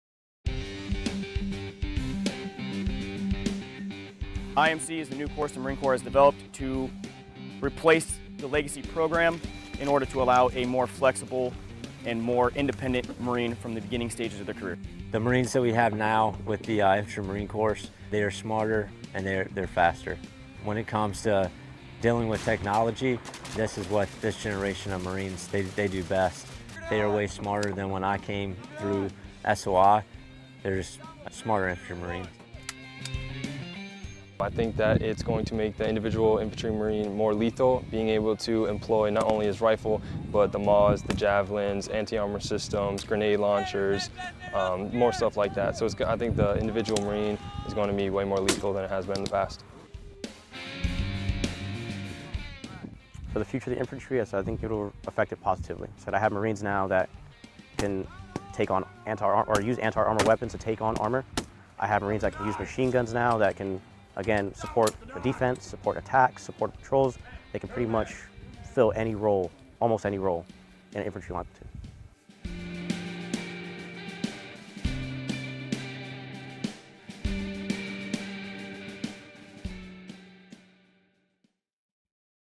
Infantry Marine Course Training
MCB CAMP LEJEUNE, NC, UNITED STATES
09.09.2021

The new Infantry Marine Course (IMC) takes place at the School of Infantry-East on Marine Corps Base Camp Lejeune, North Carolina, Sep. 3, 2021. The IMC is a 14-week program of instruction designed to modernize infantry training by improving efficiency and lethality. (U.S. Marine Corps photo by Lance Cpl. Joel Castaneda)

Film Credits: Video by Lance Cpl. Joel Castanedarodr
Marine Corps Installations East